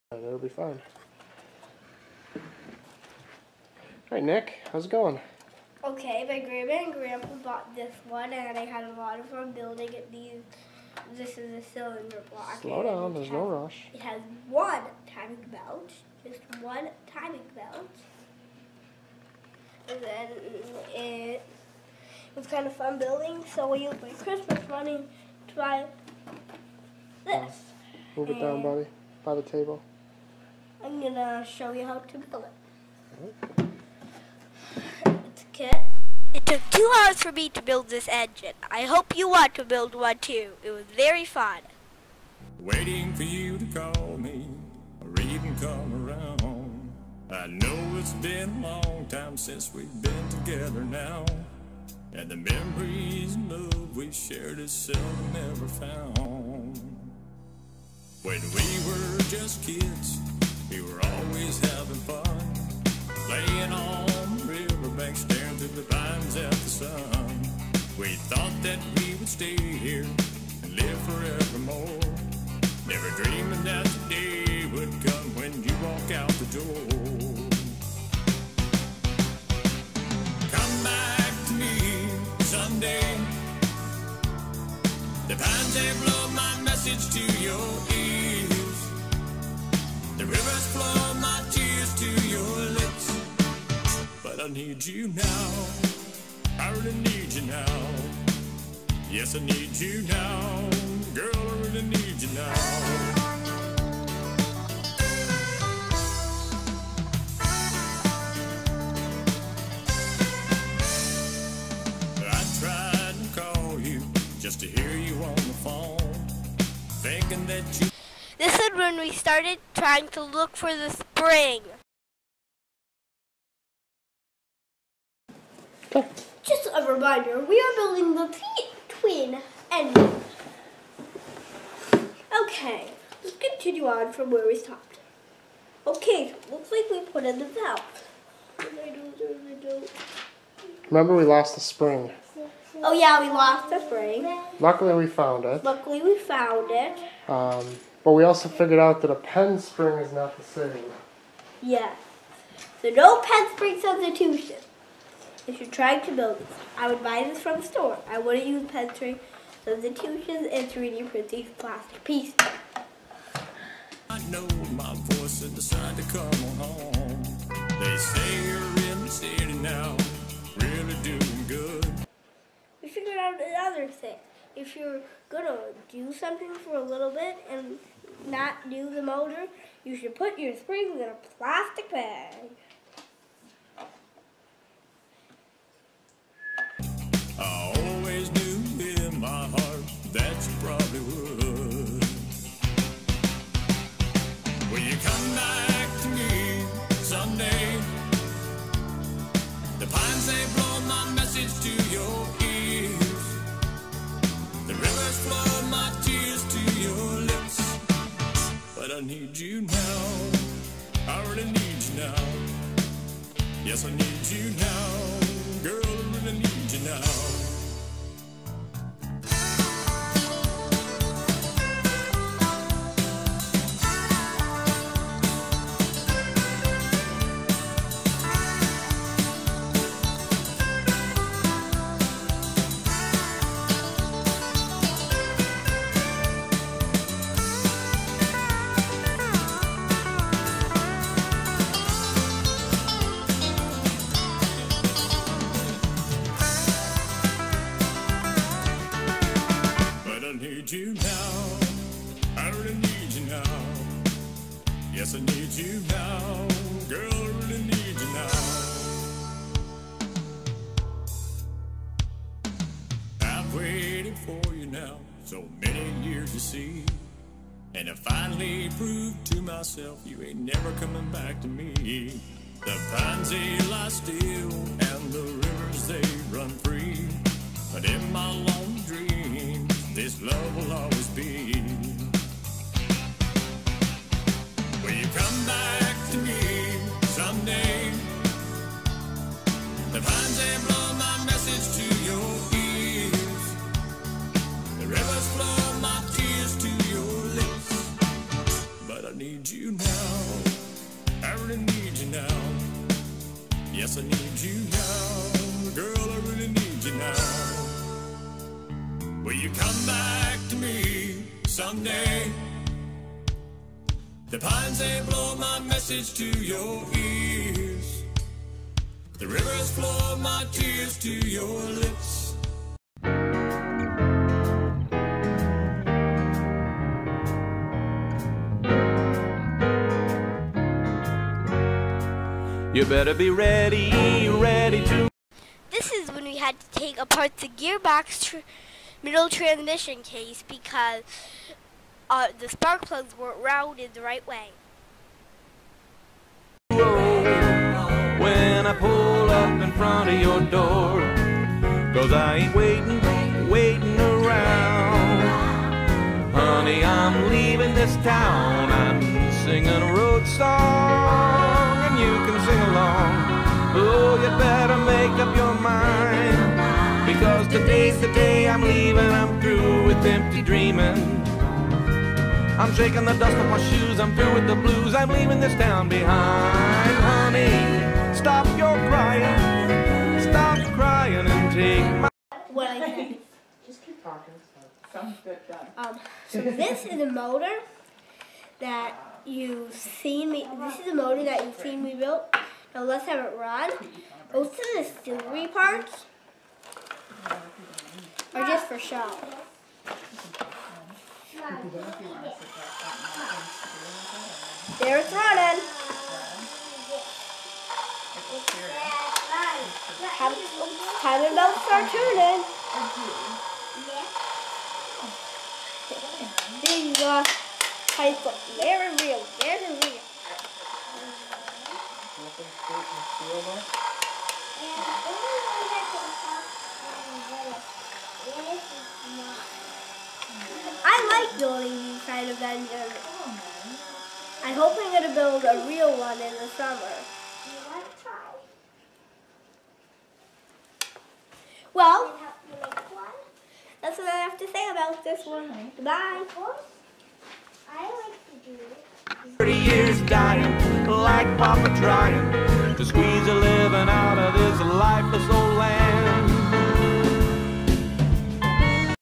Building the Hayes V-twin Motorcycle Engine
7 year old building a model of a motorcycle engine